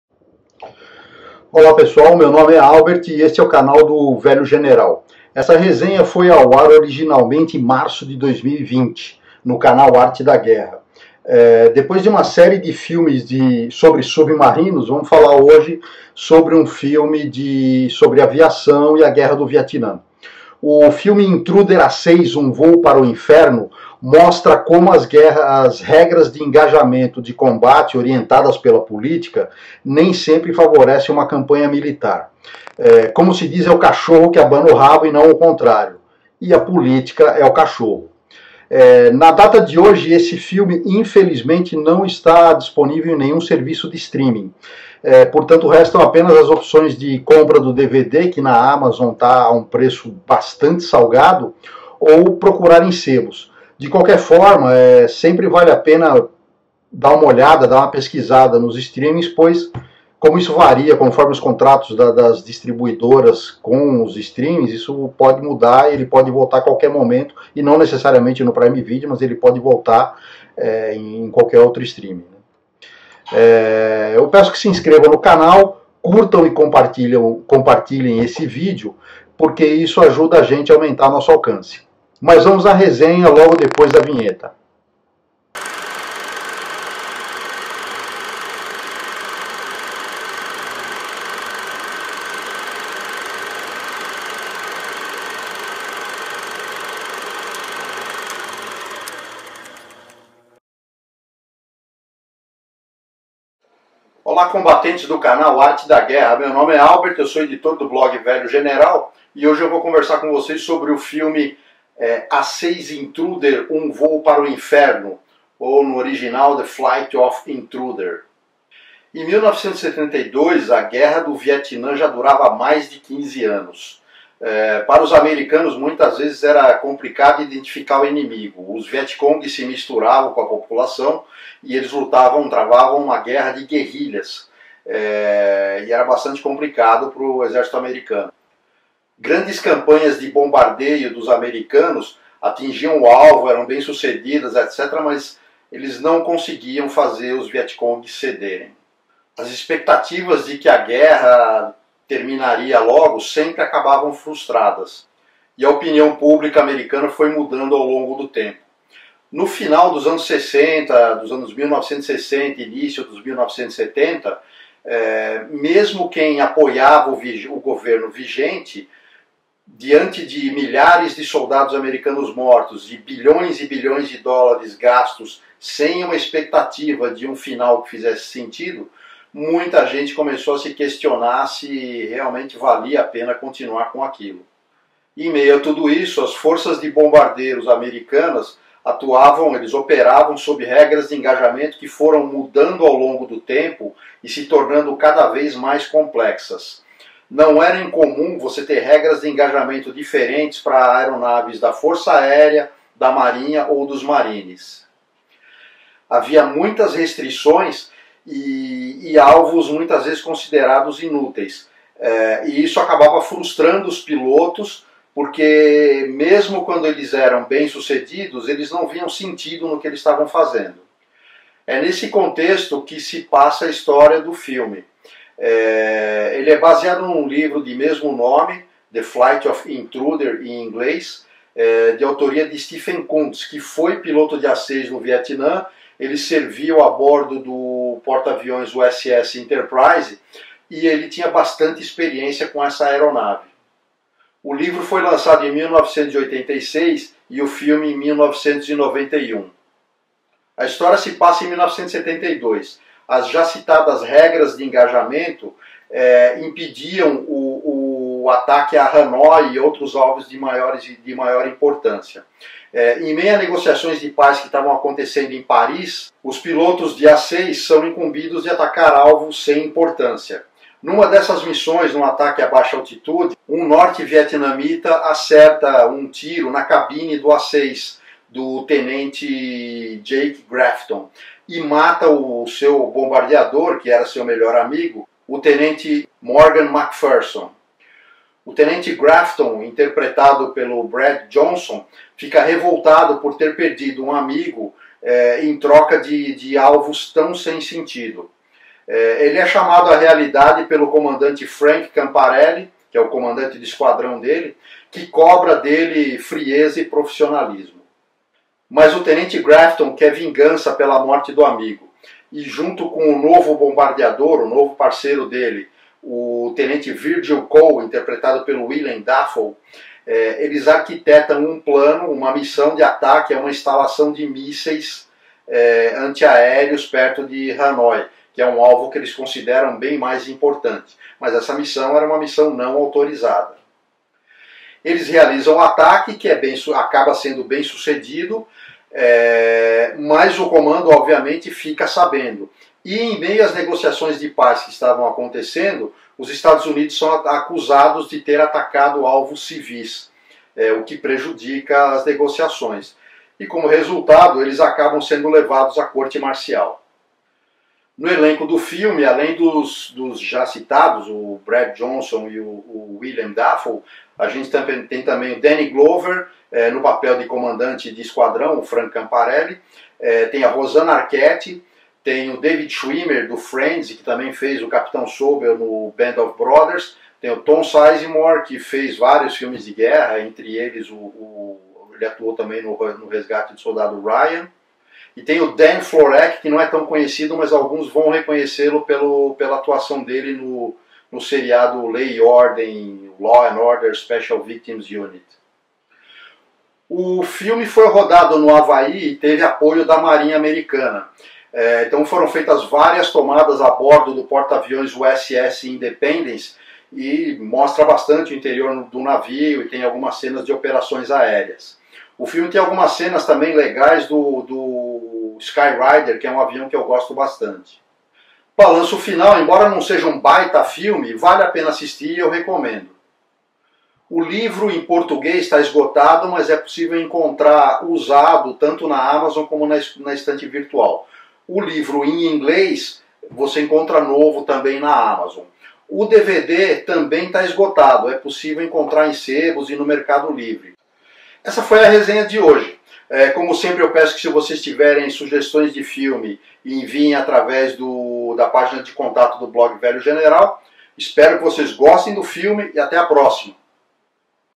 Resenha 010: Intruder A-6 Um voo para o inferno
Resenha do filme Intruder A-6 Um voo para o inferno, que se passa na Guerra do Vietnã.